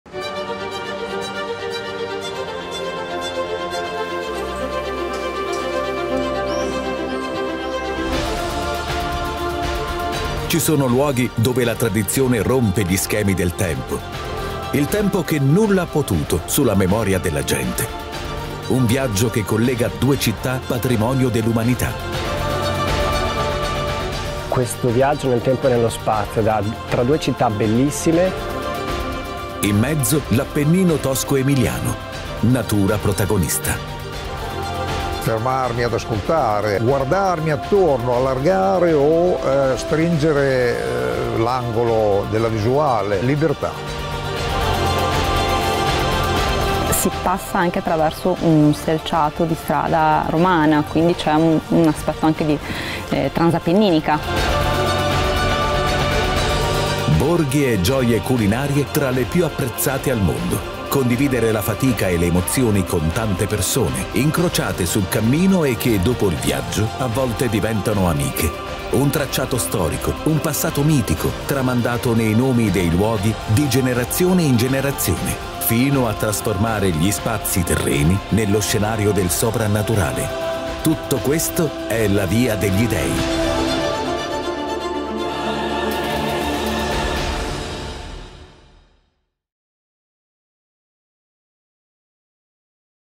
La Via degli Dei
🇮🇹 In cammino lungo la Via degli Dei, antica strada Romana di collegamento tra Bologna e Firenze, che deve il suo nome ai suggestivi toponimi dei monti che attraversa: Monte Adone, Monte Venere, Monte Giunone.

🇬🇧 Walking along the Via degli Dei, the ancient Roman road between Bologna and Florence, which owes its name to the evocative toponyms of the mountains that it crosses: Monte Adone, Monte Venere and Monte Giunone.